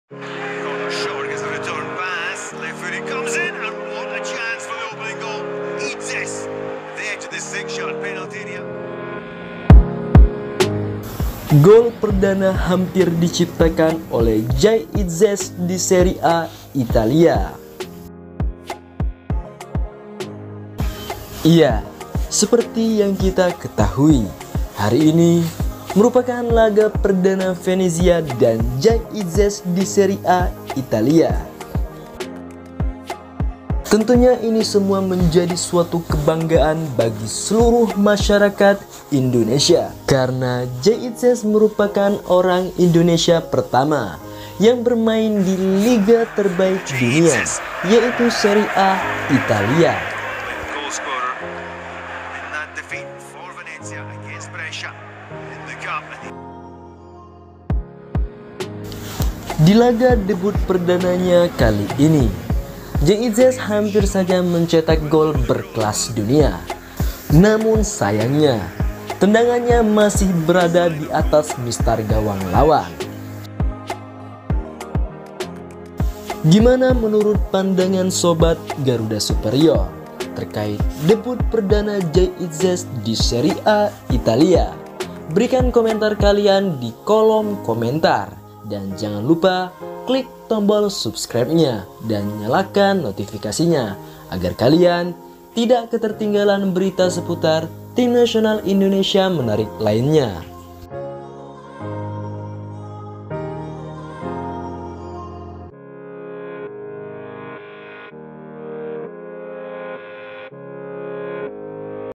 JAY IDZES HAMPIR CETAK GOAL DI SERIE A ITALIA‼️ DEBUT PERDANA JAY IDZES DI SERIE A
Seputar Timnas Indonesia, Olahraga sepakbola, pemain keturunan Indonesia